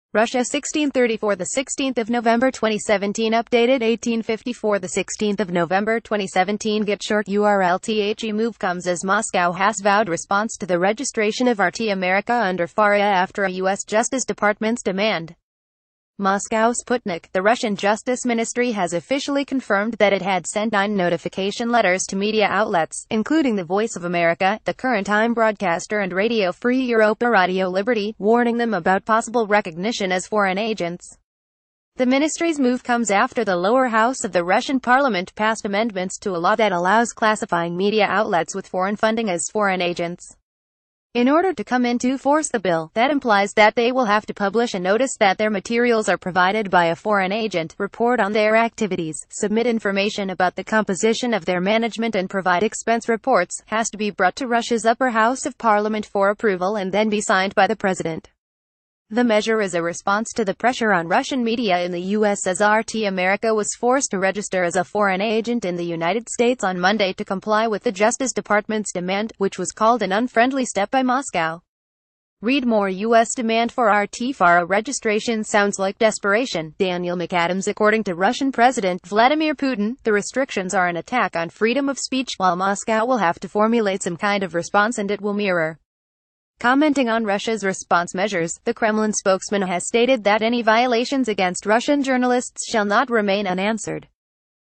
GREAT US - NEWS - The Russian Ministry to notify the media agent 9 status can have the foreign agent
GREAT US - NEWS - GREAT US - NEWS - The Russian Ministry to notify the media agent 9 status can have the foreign agent

Russia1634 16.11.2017updated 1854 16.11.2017 Get short URLThe move comes as Moscow has vowed response to the registration of RT America under FARA after a US Justice Departments demand. MOSCOW Sputnik — The Russian Justice Ministry has officially confirmed that it had sent nine notification letters to media outlets, including the Voice of America, the Current Time broadcaster and Radio Free EuropeRadio Liberty, warning them about possible recognition as foreign agents. The ministrys move comes a...

© GREAT US - NEWS